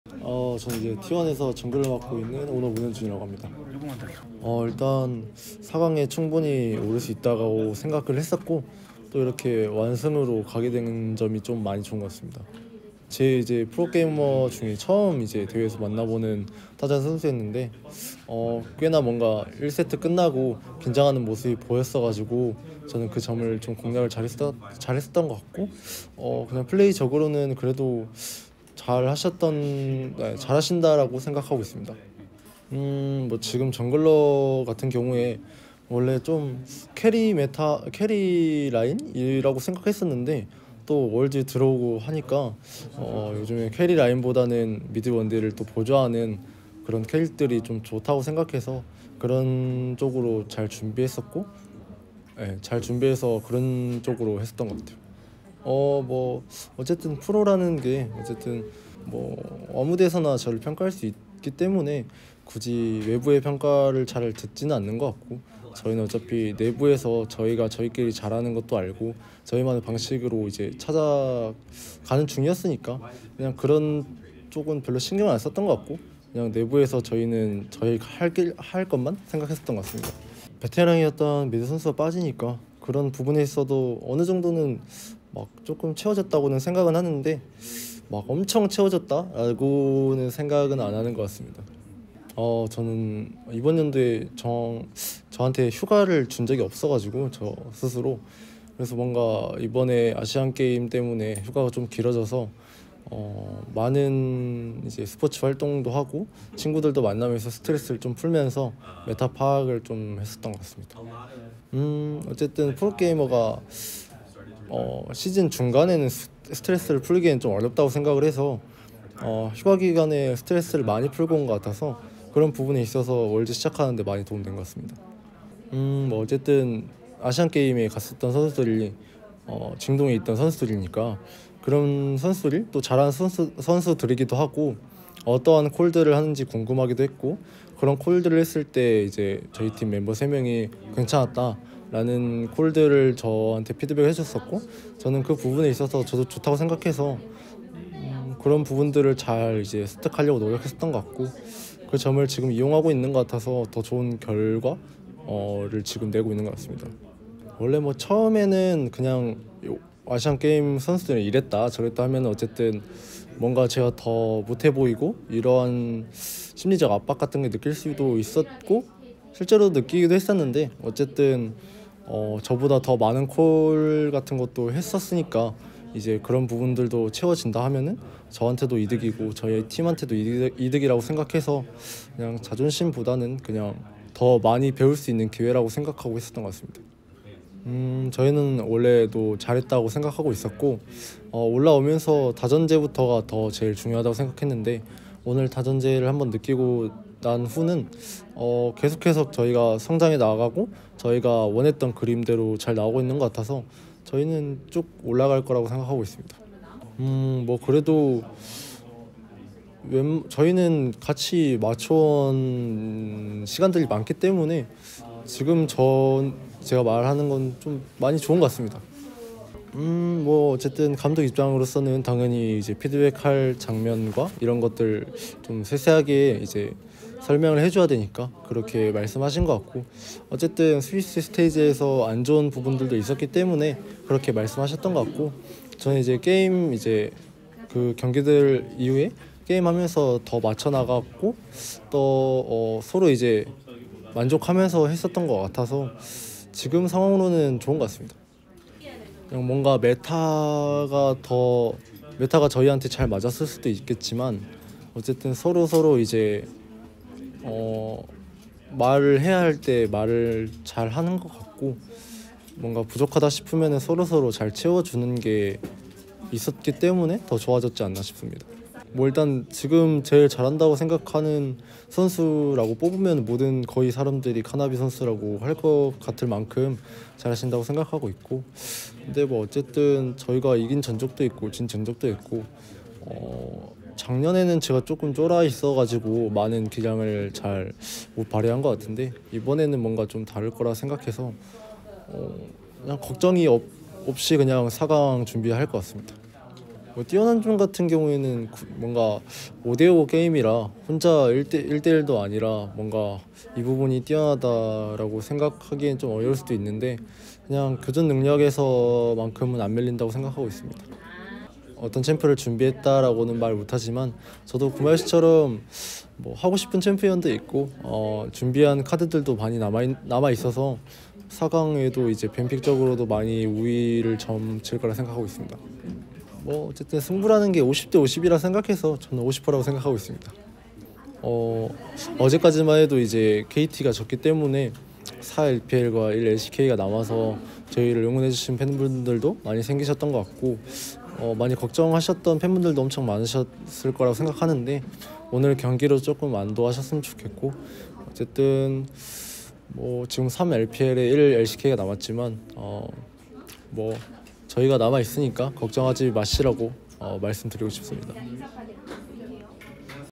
롤드컵 위해 자존심까지 버린 T1 오너 "카나비와 대결 할만하다"
2023년 11월 5일 리그 오브 레전드 월드 챔피언십 8강 T1 대 LNG 승자 '오너' 문현준 인터뷰